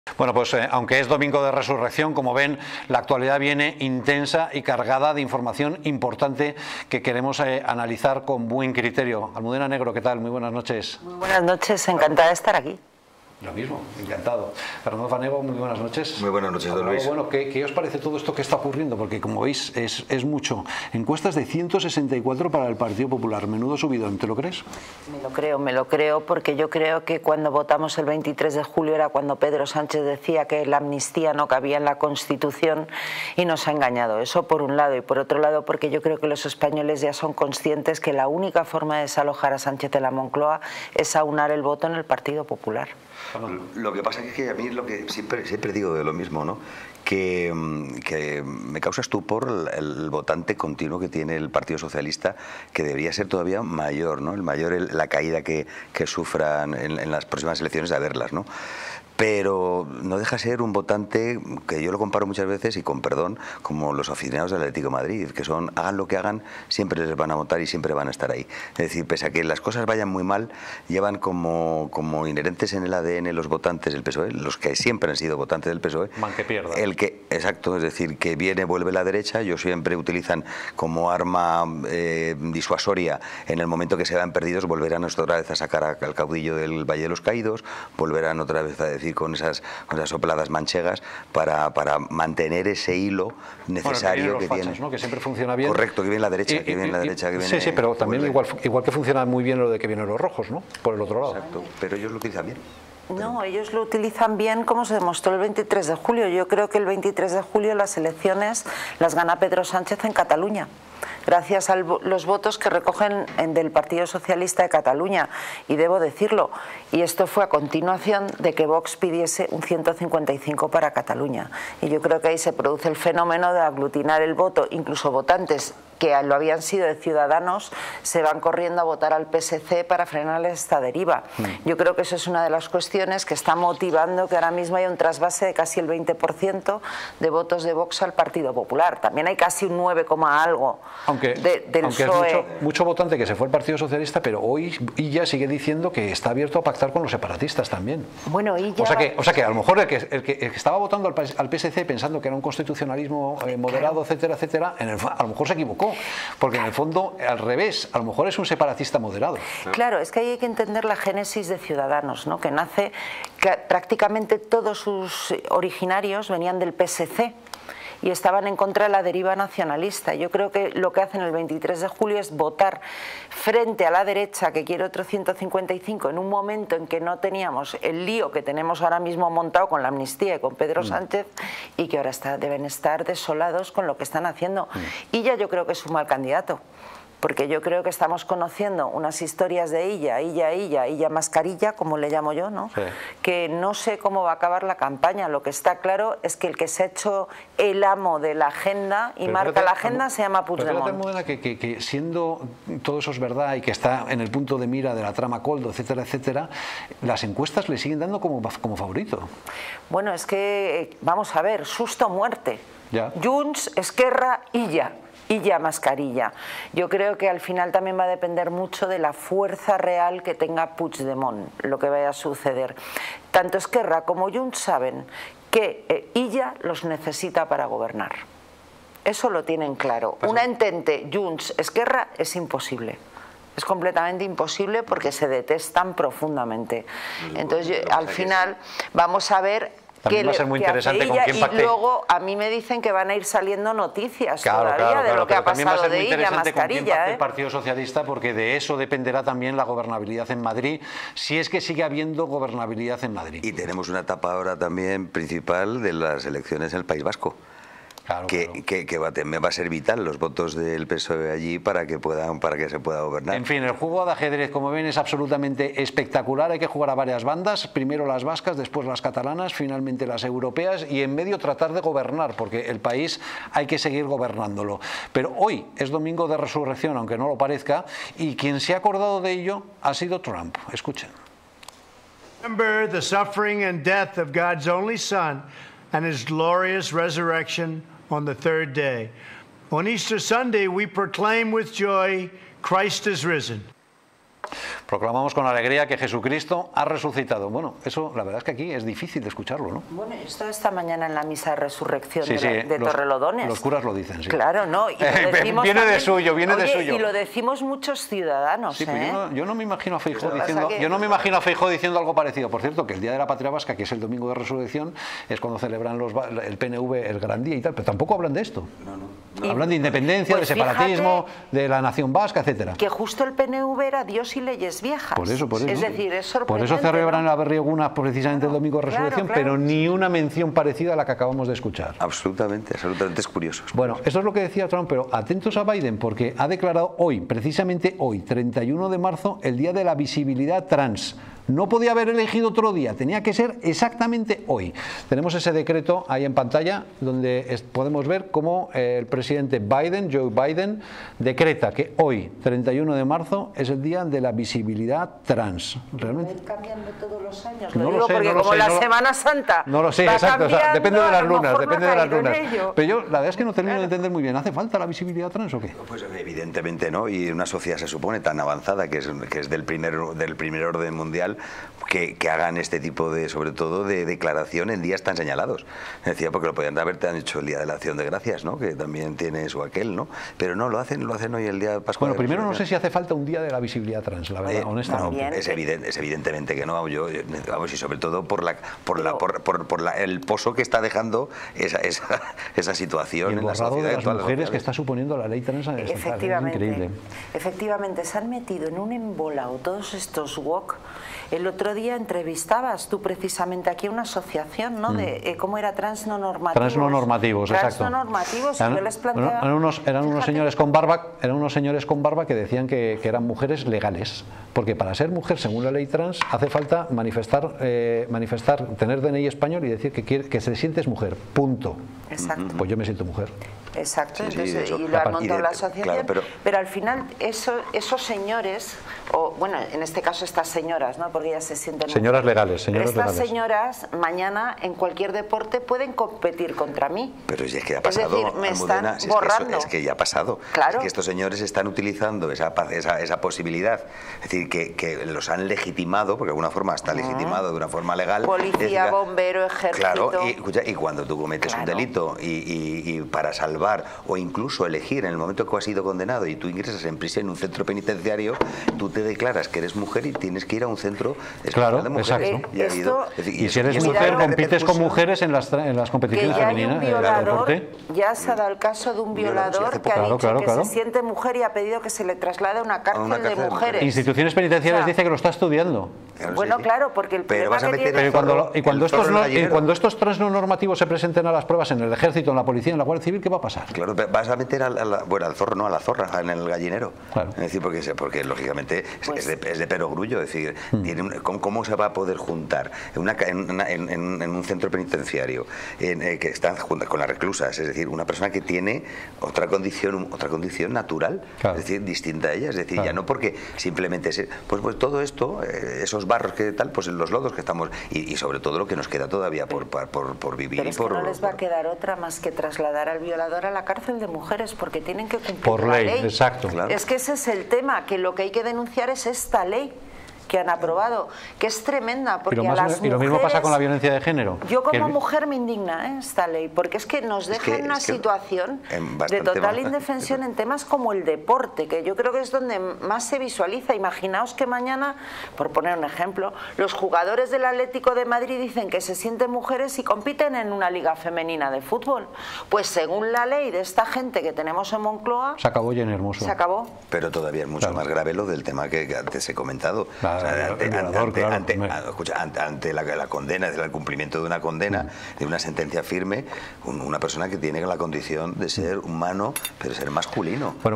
🔴SÁNCHEZ NO PARA DE CAER EN LAS ENCUESTAS Y CADA DÍA QUE PASA, EL CASO BEGOÑA LO ESTRANGULA MÁS🔴
No te pierdas las últimas noticias sobre el caso Begoña y cómo afecta a la caída en las encuestas de Sánchez. Mantente informado en Distrito TV.